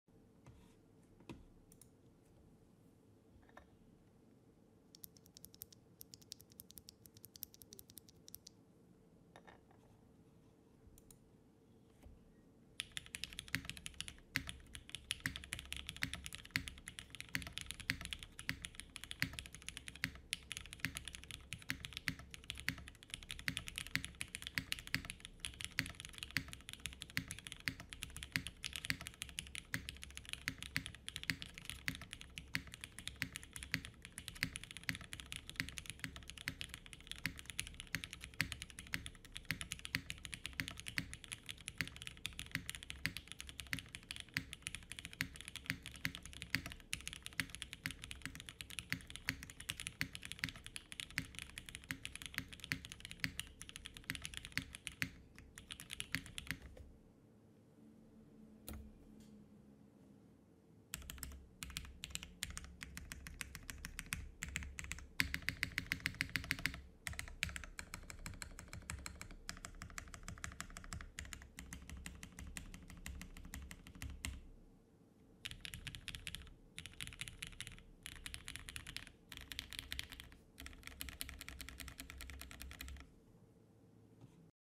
IU v2 TKL Prototype by Perry | Soundtest | Plate Polypropylene
case : IU v2 TKL Prototype by Perry
plate : Polypropylene
switches : Cherry Hyperglide Black L + F ( 205g0 and deskey films )
Spring: Tx Long 55g lube g105
keycaps : GMK  Frost Witch
stabilizers : Staebies